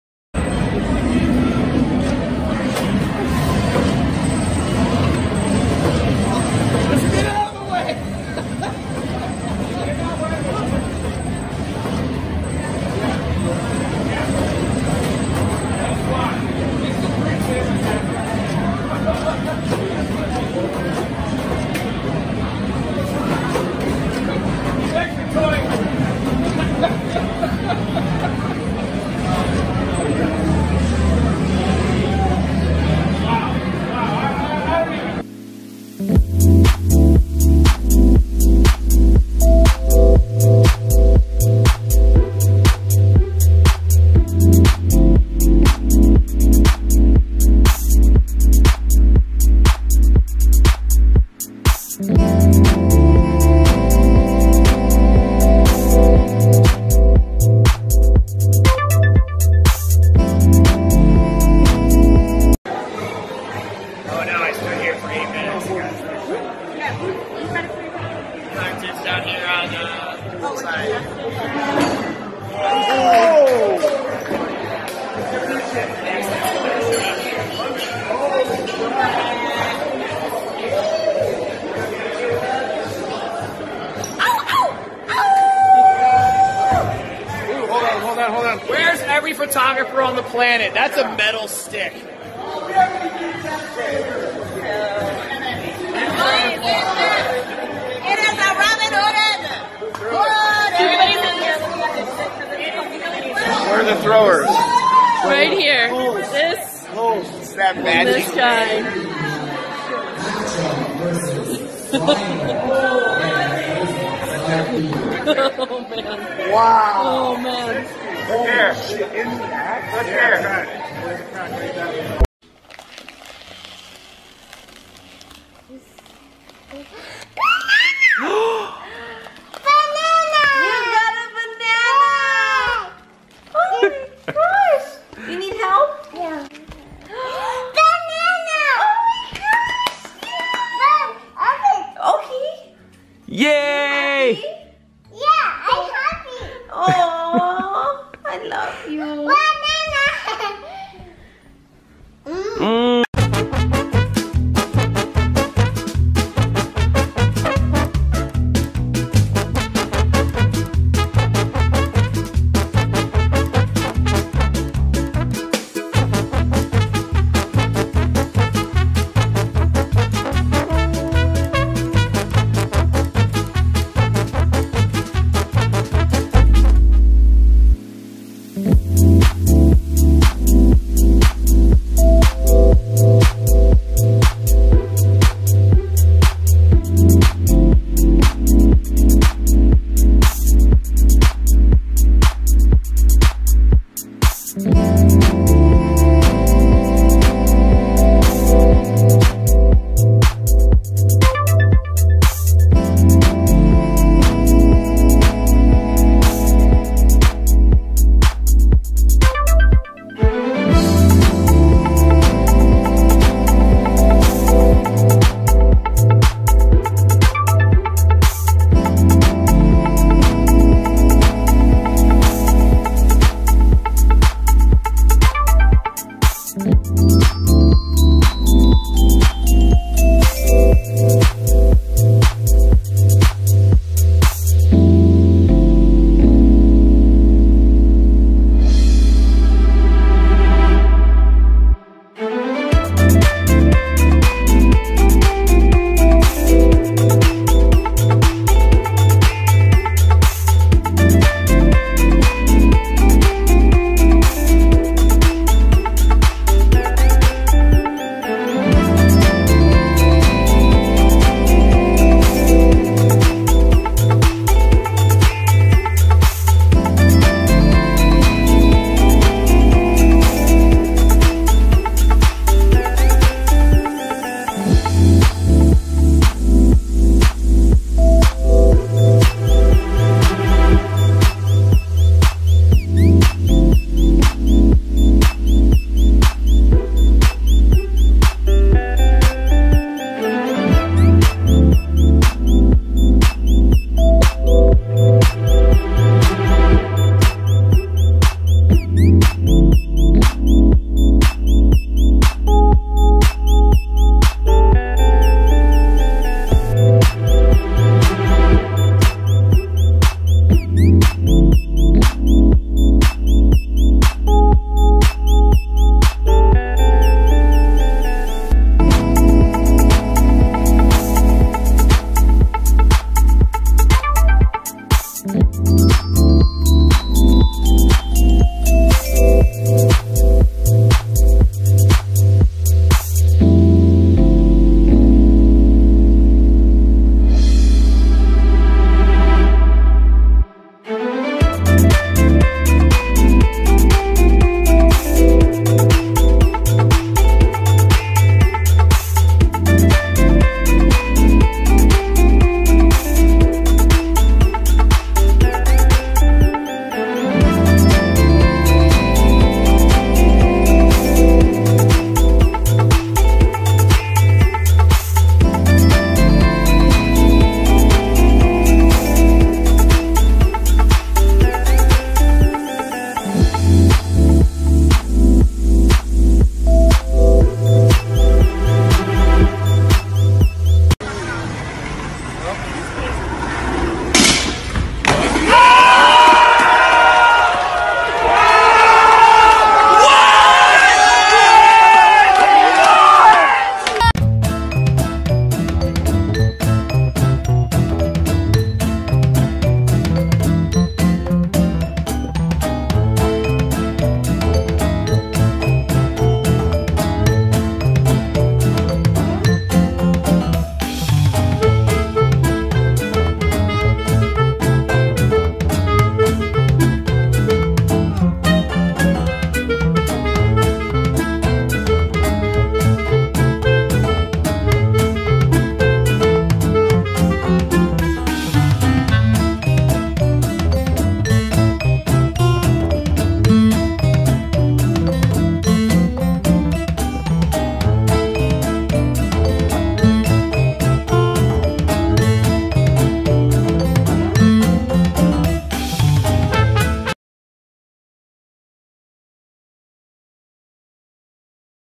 Cool Videos I found on Reddit
A different compilation of videos from Reddit that I found to be cool. Completely random. Hope you enjoy